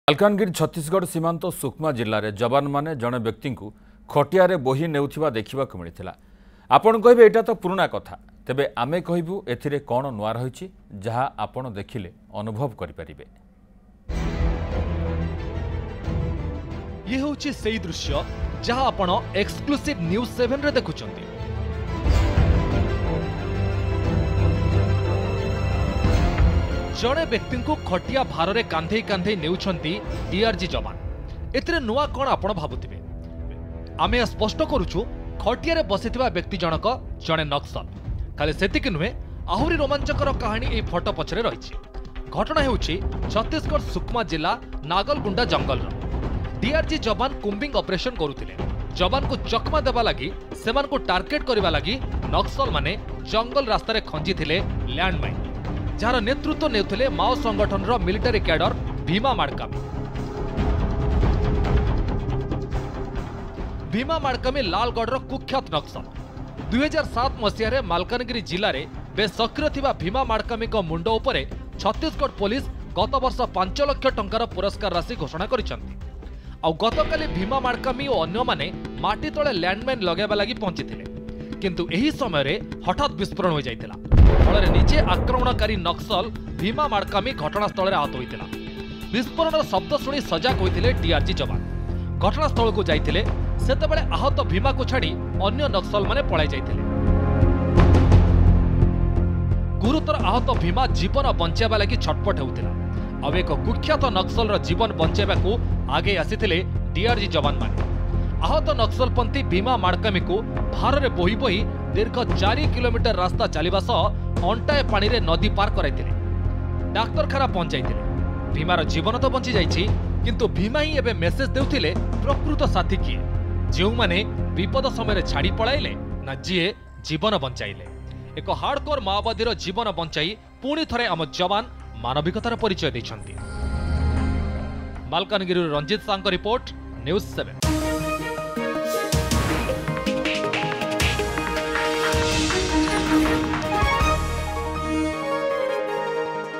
Jawans Save Life Of Maoist & Carried Him On Shoulder To Hospital
Jawan Rescued And Saved A Maoist In A Mishap While Combing Operation In Malkangiri